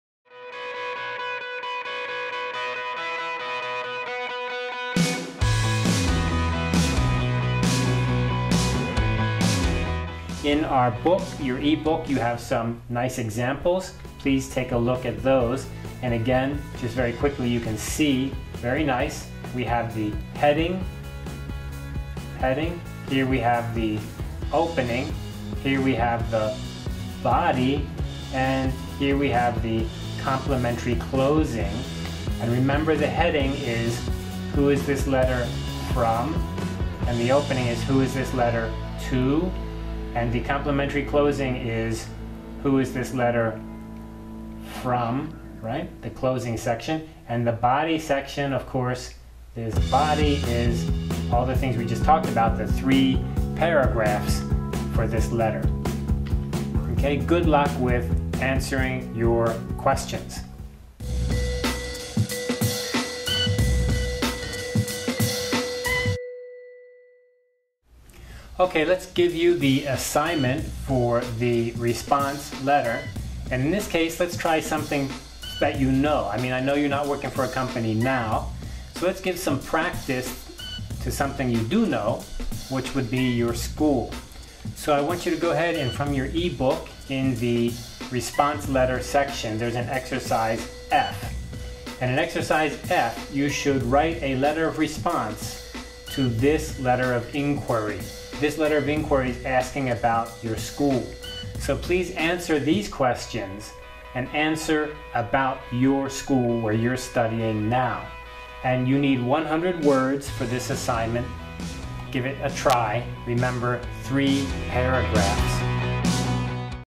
回復信例子 (Response Letter Examples)
在我們的電子書中，有幾個不錯的例子，讓我們來看看。快速地看一下信的結構，你會發現這邊是 heading, 而這裡是 opening. 這裡是文章內容， 這裡則是結尾敬語。請記得 heading 寫的是寄件方資訊，opening 是收件方資訊，而結尾敬語則是寫上寄信人，對嗎？內文部分，當然,就是我們剛剛談到的那三段。